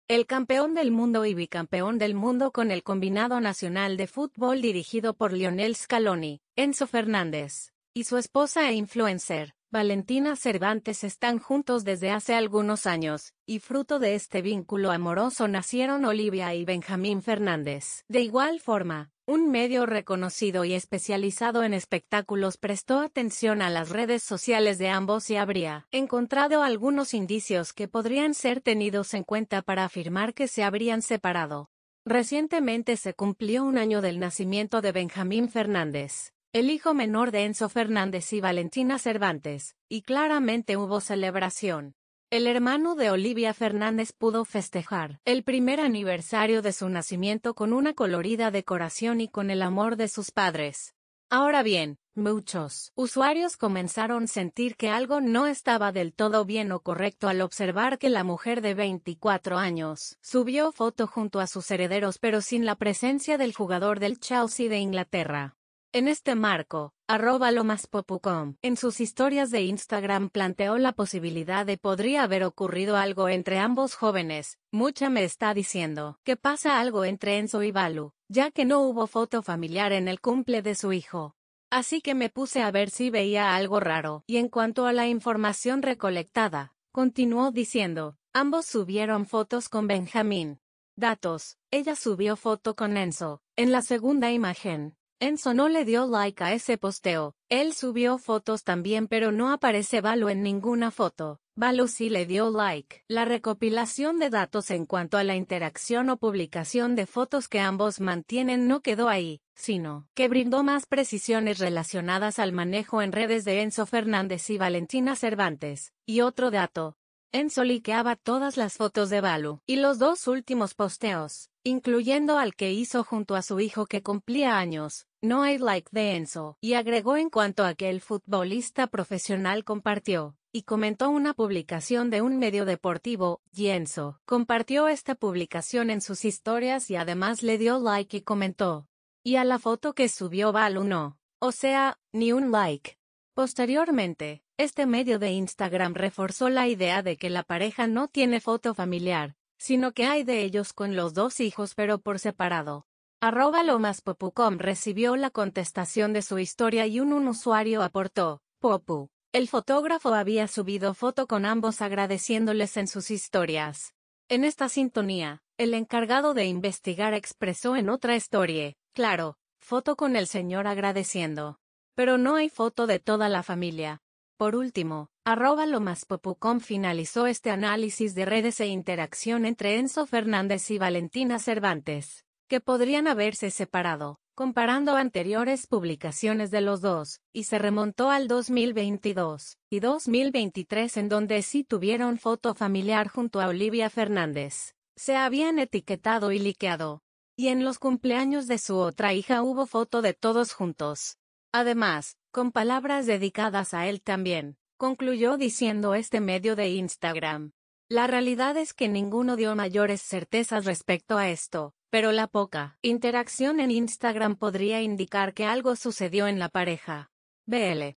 Revelaron pruebas que indicarían que Enzo Fernández y Valentina Cervantes estarían separados: l...
Revelaron pruebas que indicarían que Enzo Fernández y Valentina Cervantes estarían separados: los detalles